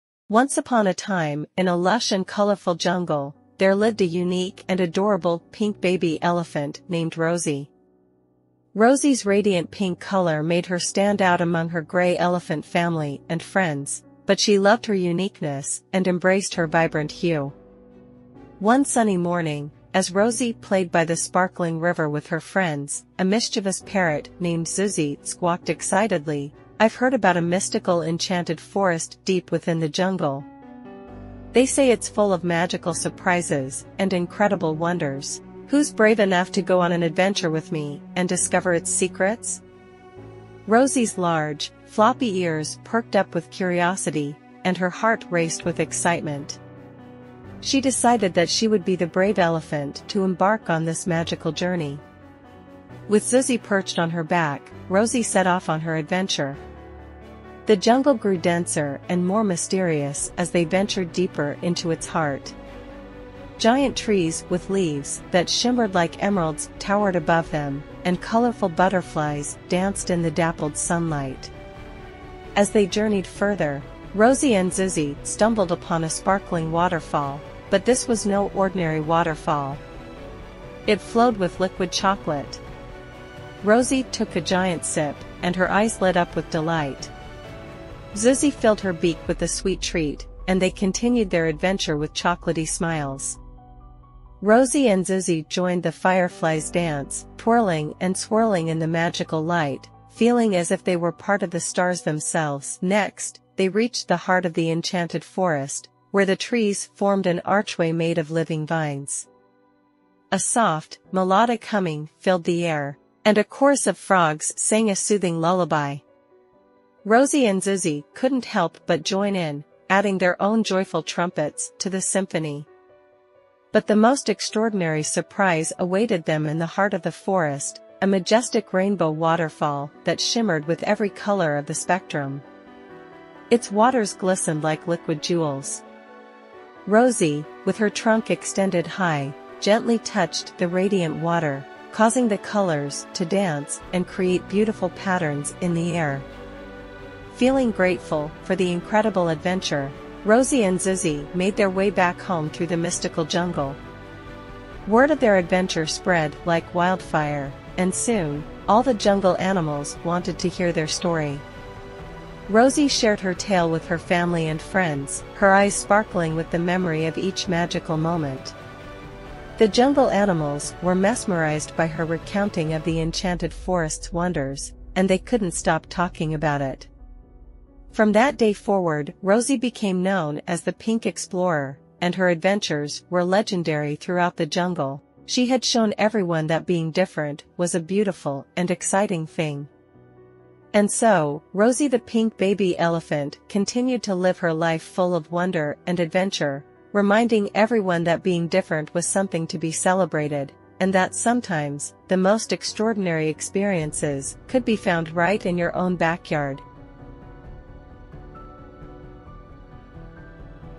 Rosie the Pink Baby Elephant's Enchanted Adventure | English Story |Bedtime Story
Join Rosie the baby elephant on an incredible adventure of a lifetime! In this heartwarming and exciting YouTube story, you'll follow Rosie as she embarks on a journey filled with curiosity, friendship, and discovery.